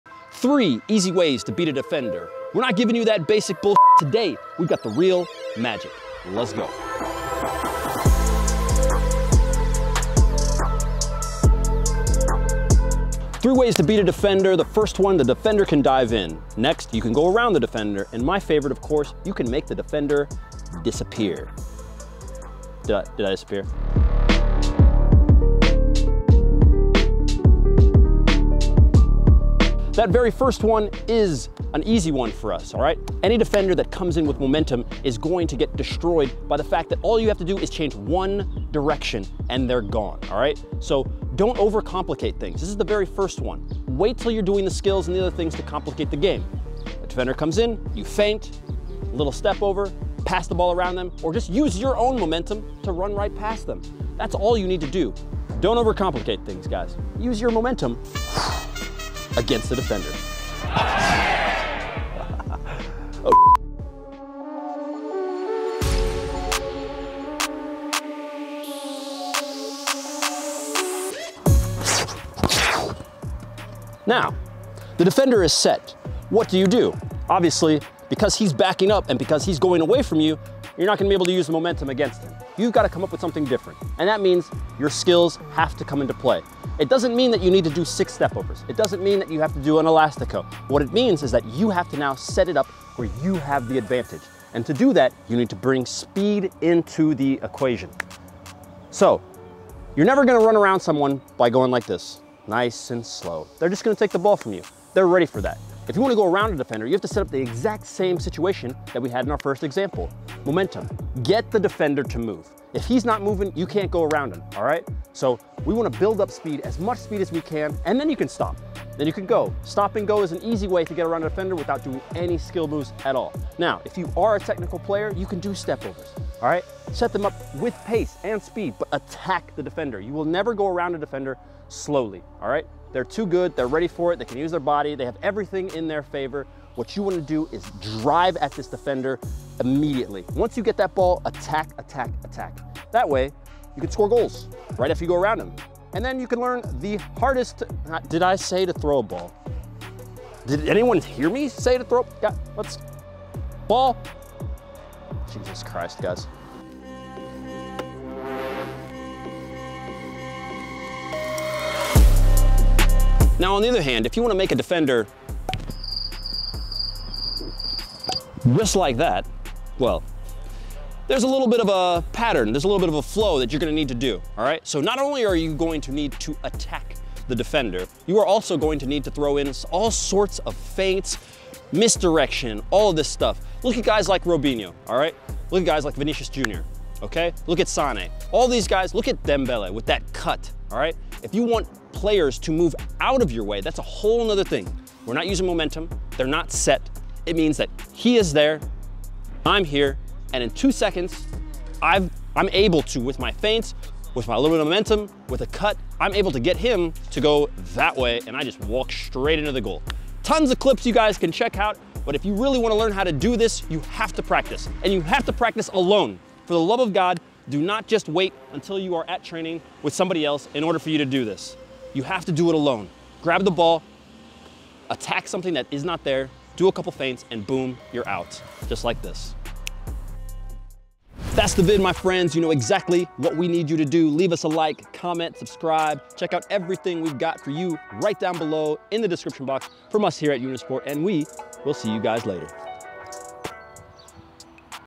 The 3 easy ways to ALWAYS beat defenders
How to beat defenders - learn football skills with Will John and Unisport. In today's football skills dribbling video, professional football player Will John takes you through the 3 golden rules that you need to learn in order to always beat defenders - so if you want to improve your dribbling, this video is a great way to start!